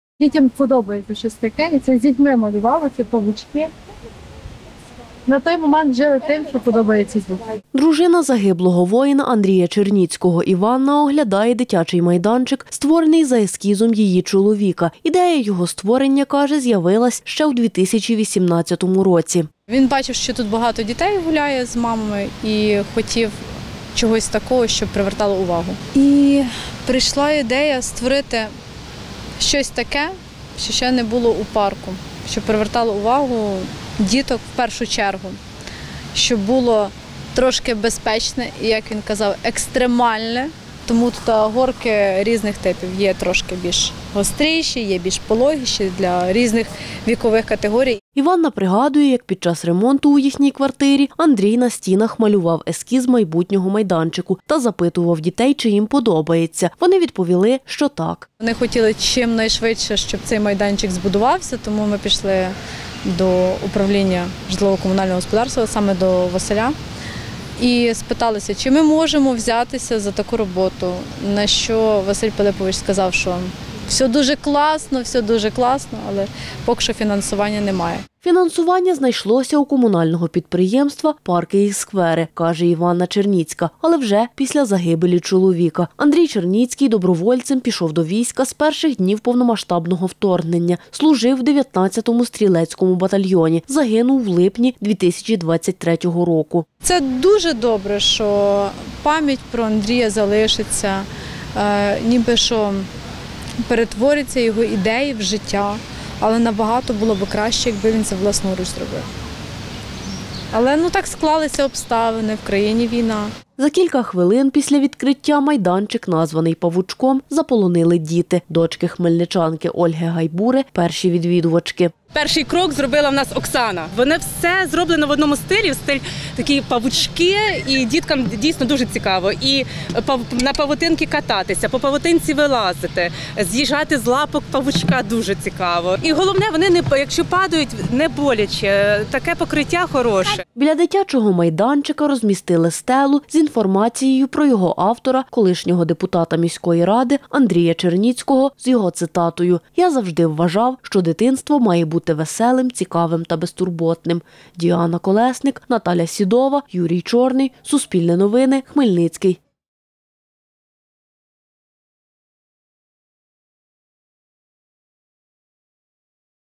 У Хмельницькому відкрили дитячий майданчик за ескізом загиблого воїна
У хмельницькому парку імені Михайла Чекмана відкрили дитячий майданчик, розроблений за ескізом полеглого воїна Андрія Черніцького. Він загинув у липні 2023 року. Ідея створення майданчика у Андрія з’явилась ще у 2018 році.